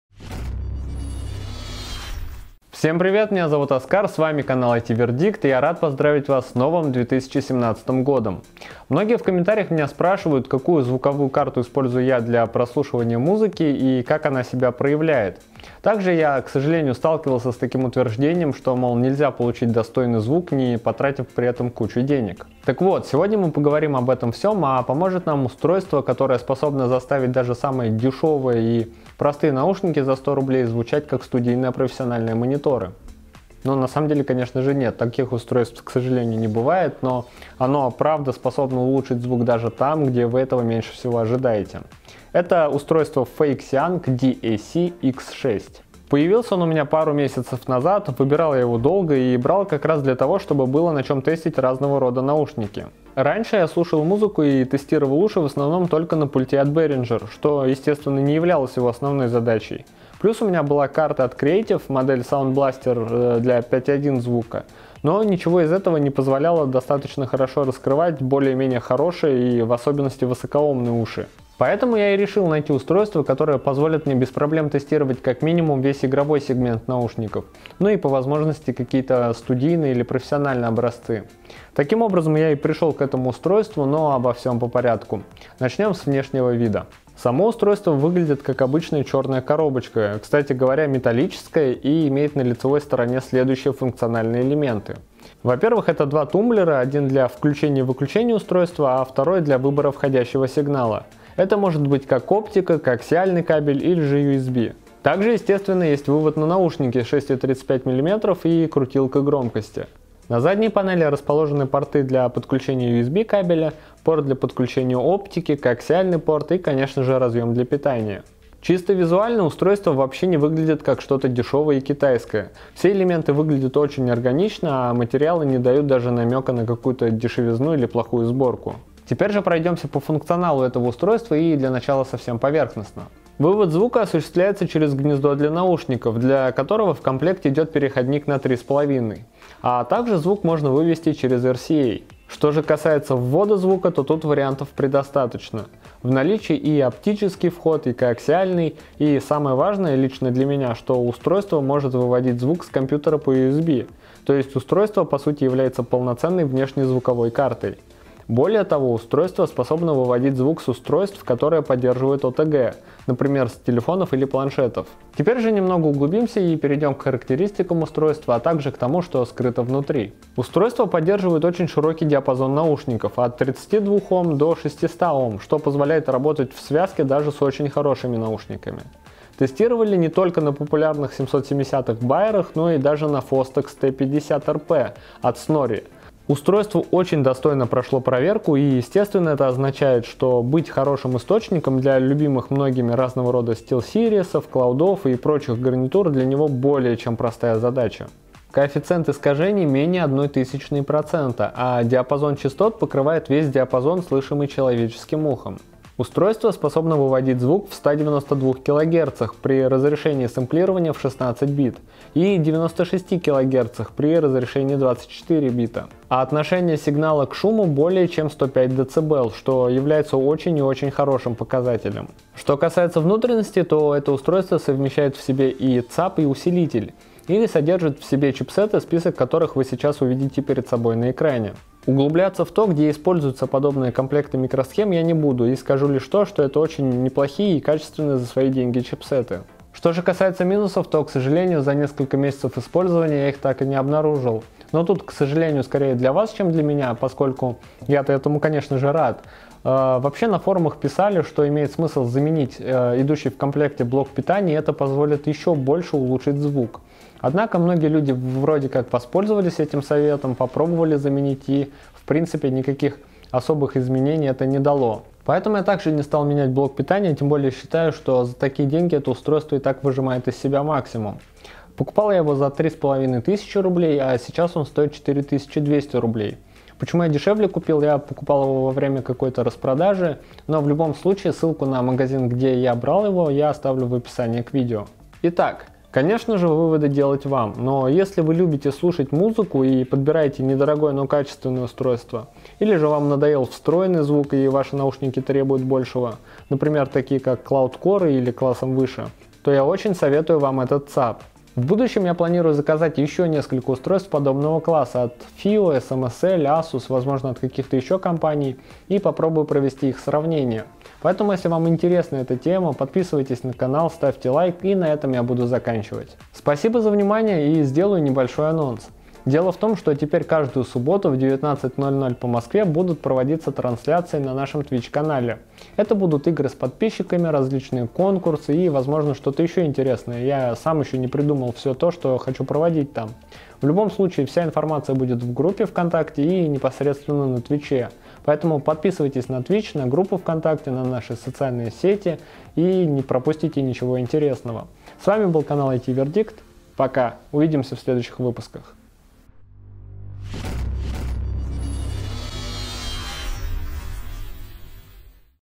Feixiang Audio DAC-X6 - отличная звуковая карта для хороших наушников
Обзор на китайский ЦАП - Feixiang Audio DAC-X6. Отличная замена недорогим звуковым картам, которая позволит выжать из вашей гарнитуры максимум.